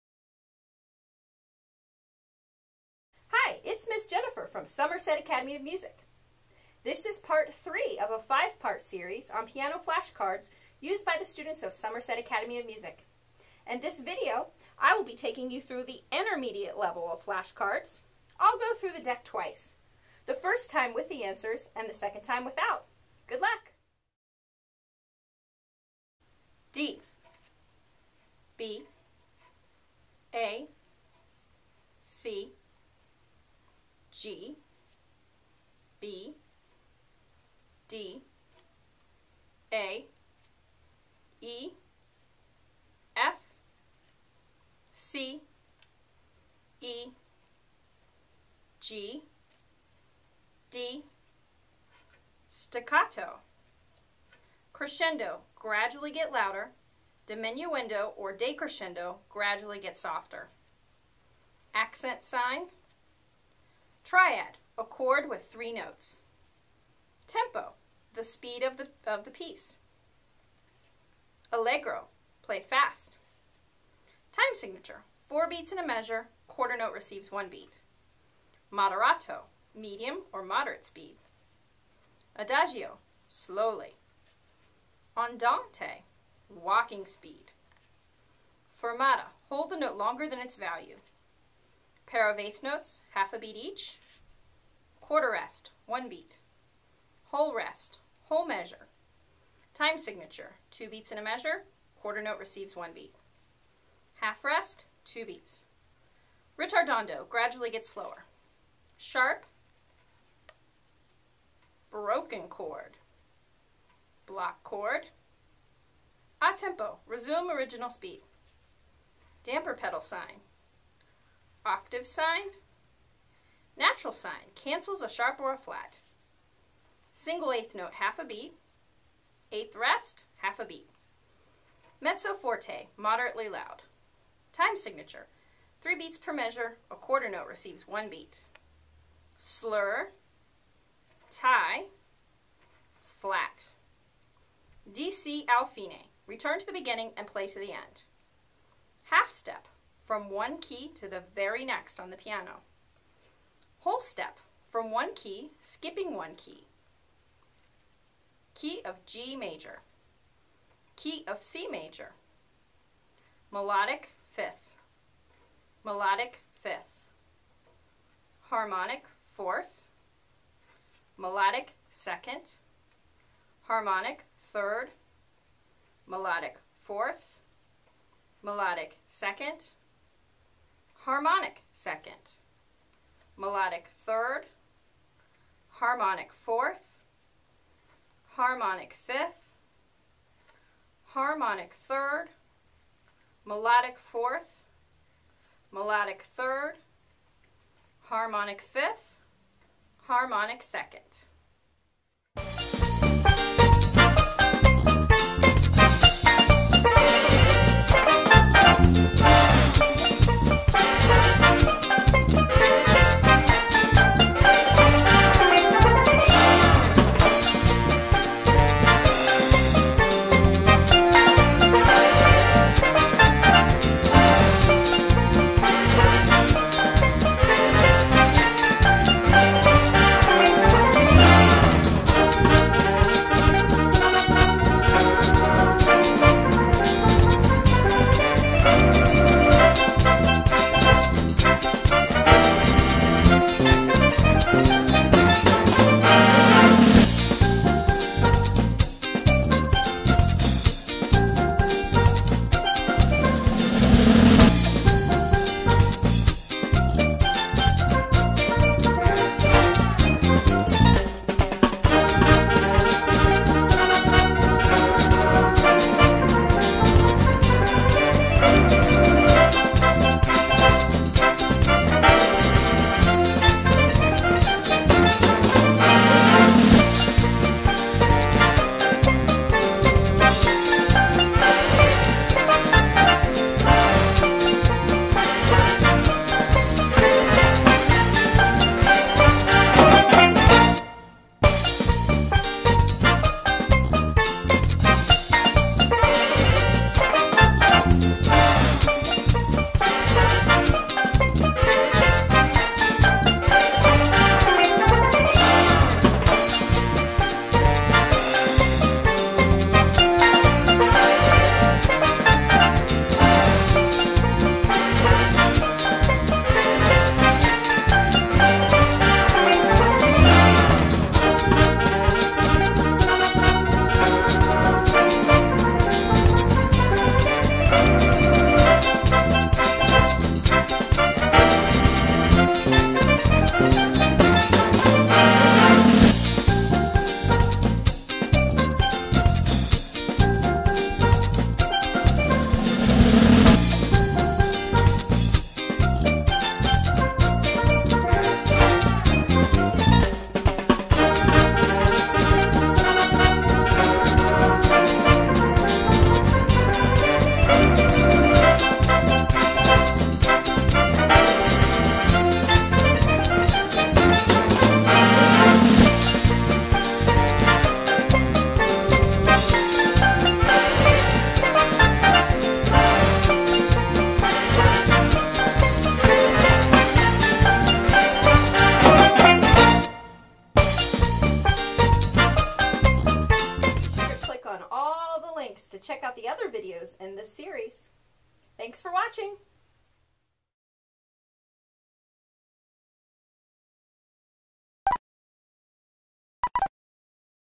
Intermediate Piano Flashcards Part 3
Somerset Academy of Music piano teacher Miss Jenifer guides you through the Intermediate Flashcards used with piano students. Music Theory flash cards are a good way to learn basic notes, symbols and terms used in music. The flash cards are from Alfred's Basic Piano Course.

    Piano Guitar & Violin Lessons
     Somerset Academy of Music